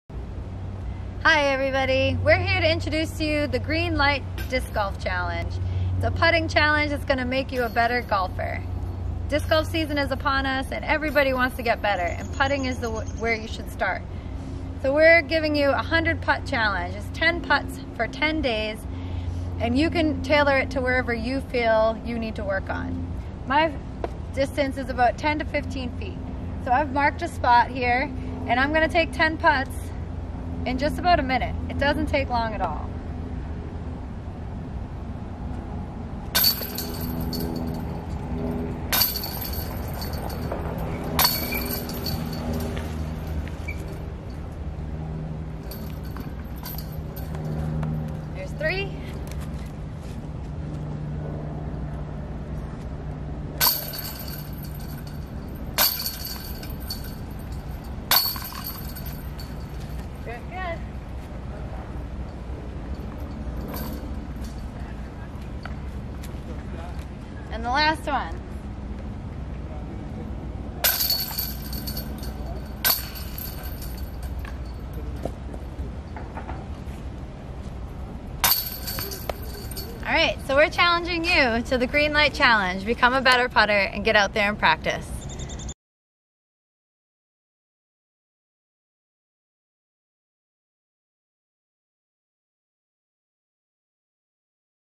Green Light Disc Golf 100 Putt Challenge
With daylight savings upon us and the disc golf season kicking off we have a way to improve your game- a quick and easy putting challenge.  10 putts for 10 days will build your confidence and lower your scores.  You decide the distance- but keep it the same for each of the 10 days and remember to track your scores to share (or to keep for yourself).  Who are you going to challenge to join you???  You could just have a putting buddy or have a friendly competition to see who comes out on top???